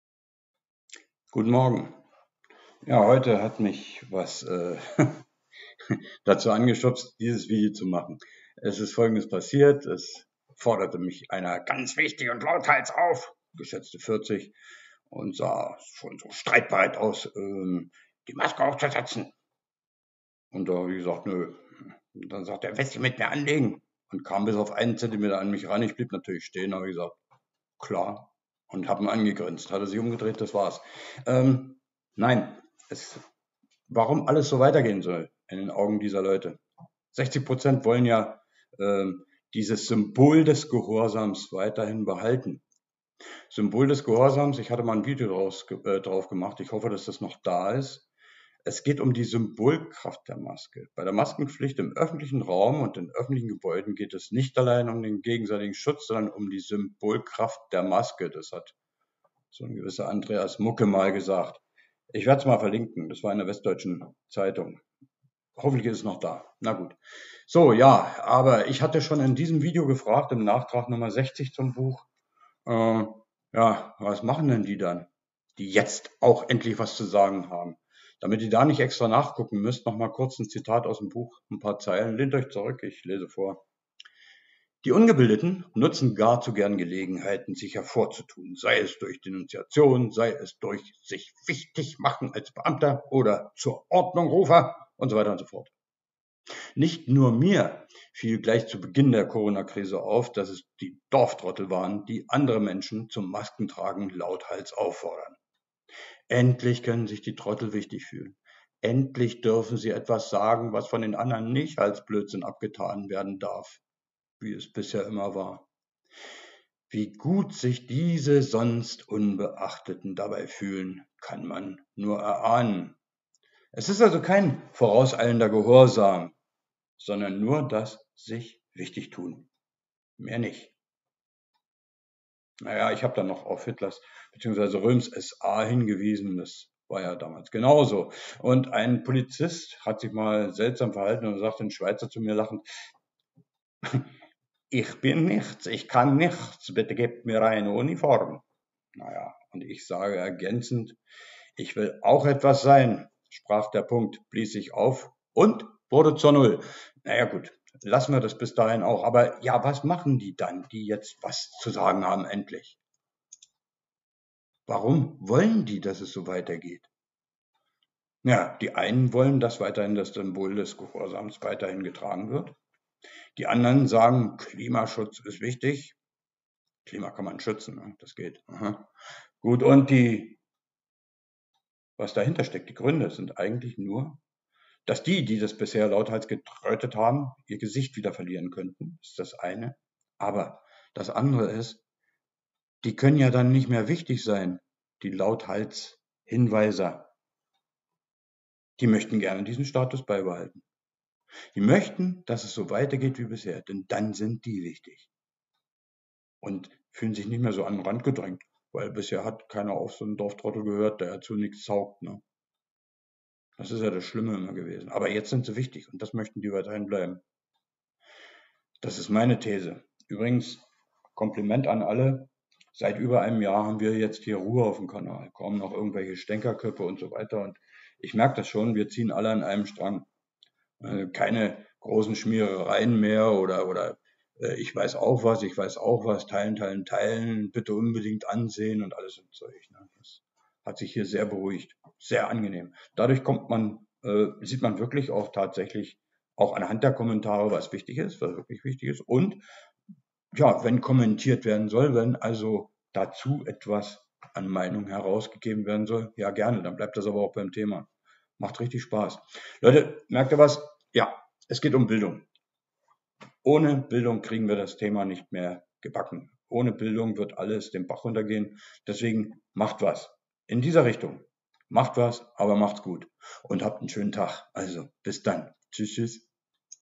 Warum für 60 % alles so weitergehen soll - Nachtrag 63 zum Buch “IQ ist nichts, ”
+++ Meinungsfreiheit im MaPhy-Forum +++ Es ist ganz einfach. Lehnt Euch zurück und hört nur zu! Ich lese die paar Zeilen vor.
Ich beziehe mich auf mein Buch; hier der

Björn Köhler